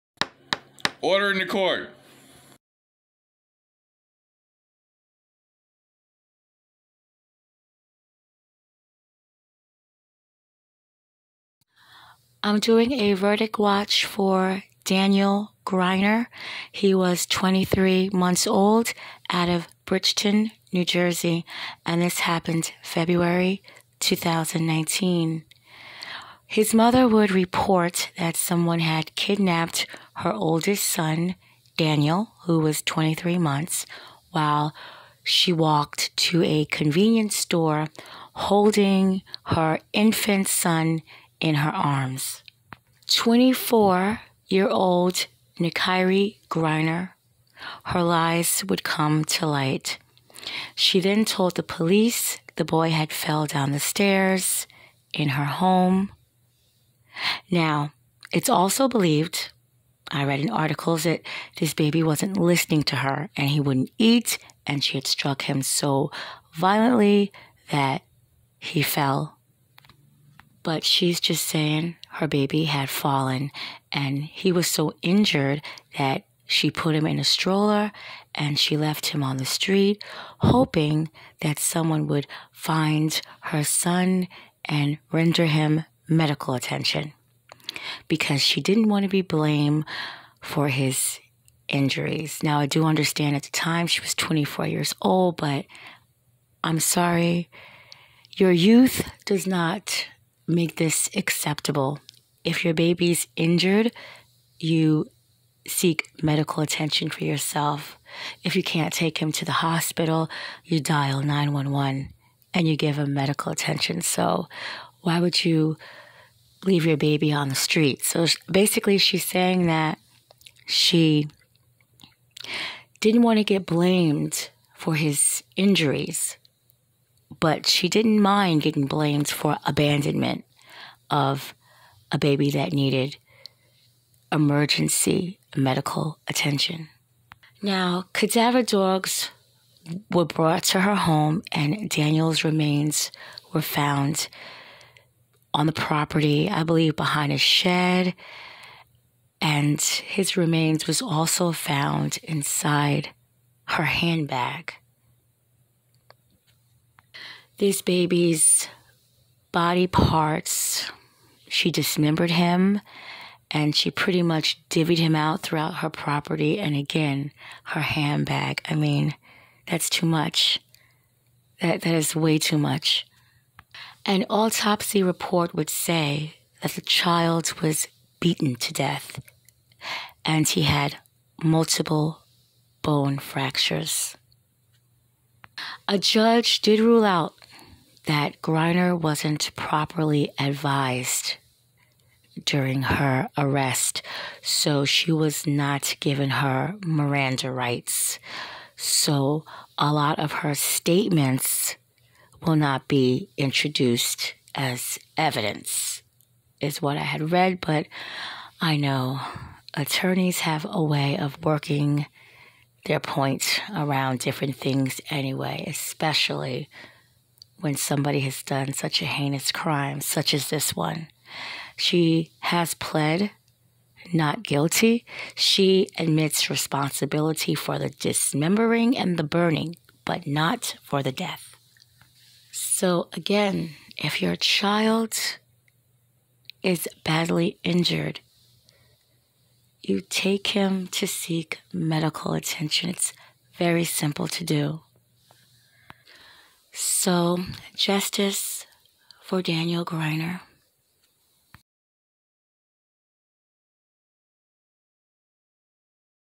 Daniel Griner(he needs justice)
Autopsy reports would read, Daniel Griner was viciously beaten to death while in his mother's custody.

Information:

Images:
Google Images

Narrator: Charlie Bacardi